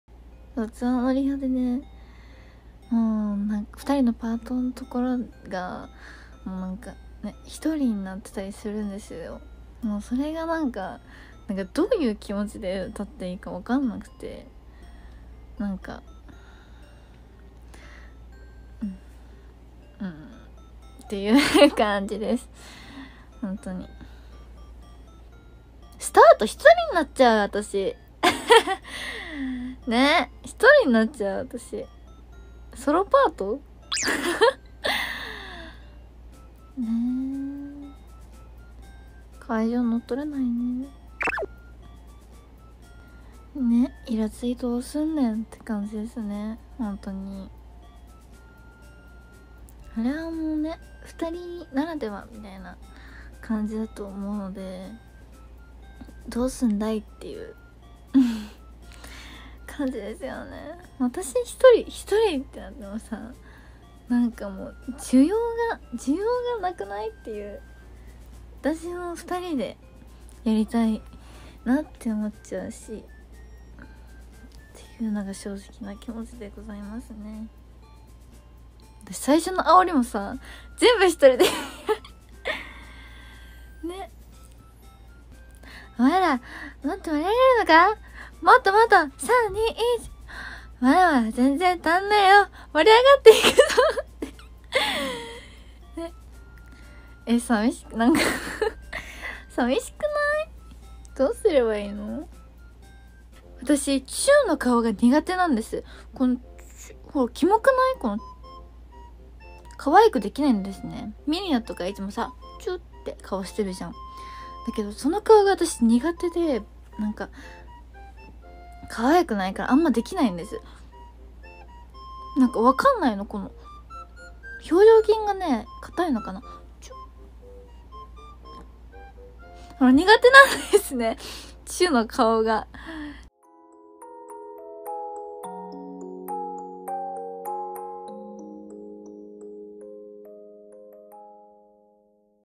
【イコラブ】齋藤樹愛羅 齊藤なぎさ卒業できあら先輩忙しくなる模様【=LOVE（イコールラブ）】
誰がなーたんの代わりやるんだろう
1:43 おまけ チュー顔

切り抜いてほしいシーンがありましたらぜひコメントお願いします。